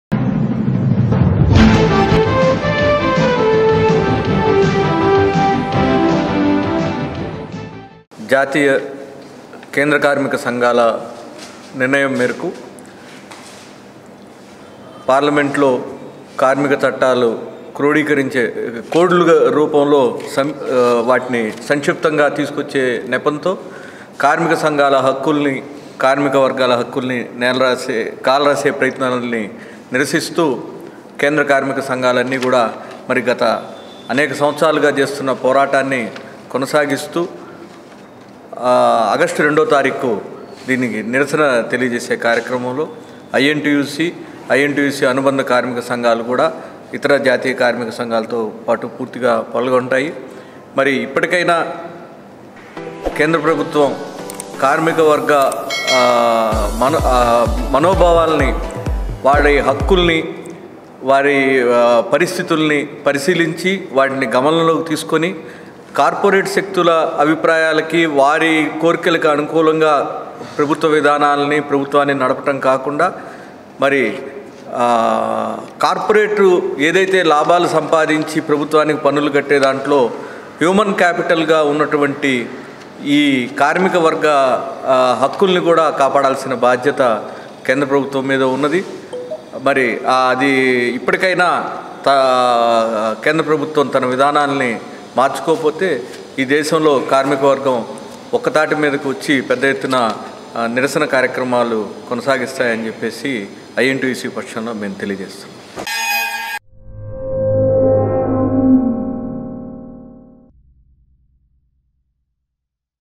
KRANTHIKUMAR కార్మిక హక్కులను కాలరాచే..కార్మిక కోడ్ ను వ్యతిరేకించాలి..
కార్మిక హక్కులను కాలరాచే..కార్మిక కోడ్ ను వ్యతిరేకించాలి.. క్రాంతి కుమార్-ఐ.ఎన్.టీ.యూ.సీ.రాష్ట్ర కార్యదర్శి